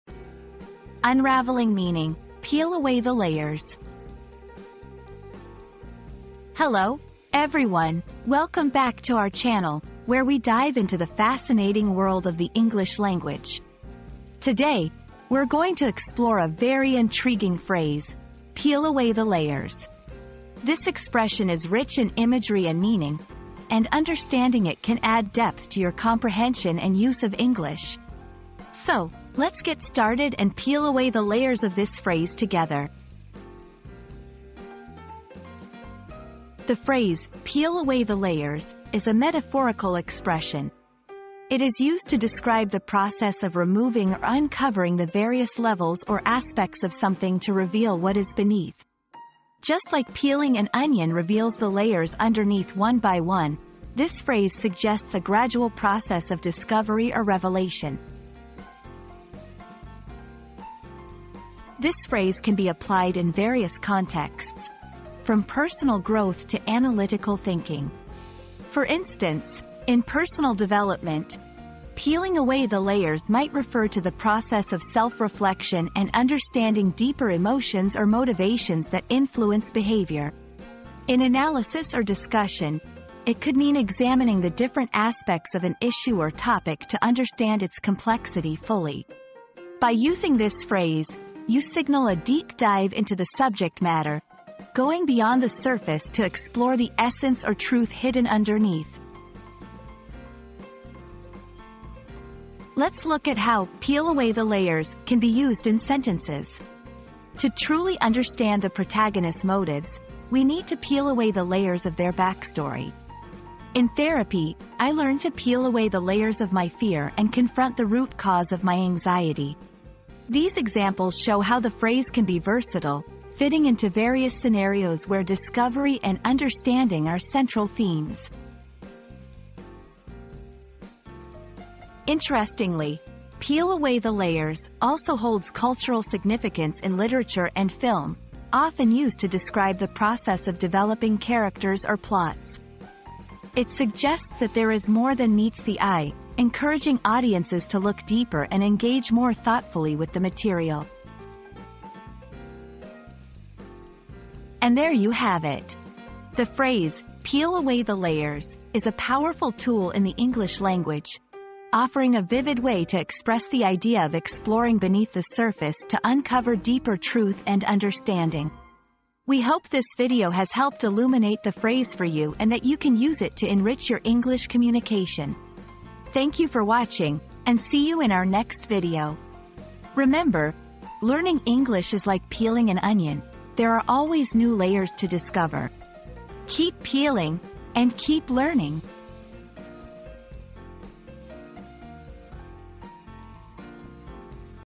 Unraveling Meaning: "Peel Away the Layers"
Unraveling Meaning: Peel Away the Layers • Join us on a journey of discovery as we peel away the layers to reveal the true meaning behind it all. Unravel the mysteries and uncover the hidden truths in this thought-provoking exploration.

00:00 • Introduction - Unraveling Meaning: "Peel Away the Layers"
00:36 • Understanding the Phrase
01:01 • Usage in Context
01:42 • Examples in Sentences
02:11 • Cultural Significance